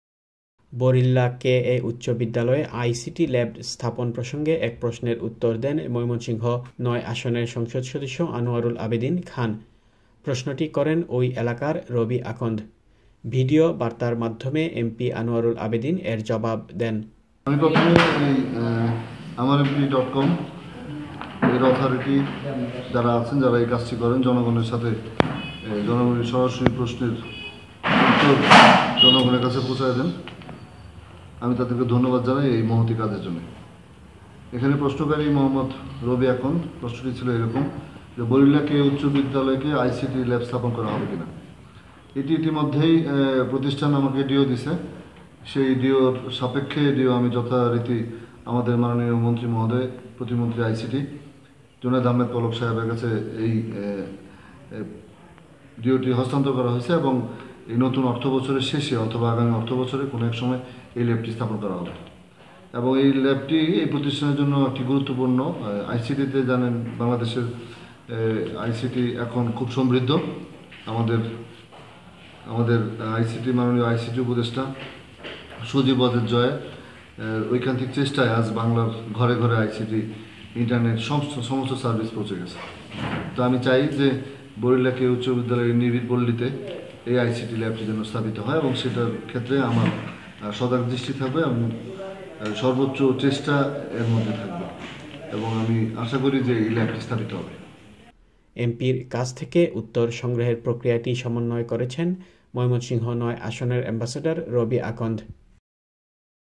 Anwarul Abedin Khan MP replied at AmarMP regarding ICT Lab
বরিল্যা কে এ উচ্চ বিদ্যালয়ে আইসিটি ল্যাব স্হাপন প্রসঙ্গে এক প্রশ্নের উত্তর দেন ময়মনসিং ৯ আসনের সংসদ সদস্য আনোয়ারুল আবেদীন খান। প্রশ্নটি করেন ঐ এলাকার রবি আকন্দ। ভিডিও বার্তার মাধ্যমে এমপি আনোয়ারুল আবেদীন এর জবাব দেন।

এমপির কাছ থেকে উত্তর সংগ্রহের প্রক্রিয়াটি সমন্বয় করেছেন ময়মনসিং ৯ আসনের এ্যাম্বেসেডর রবি আকন্দ।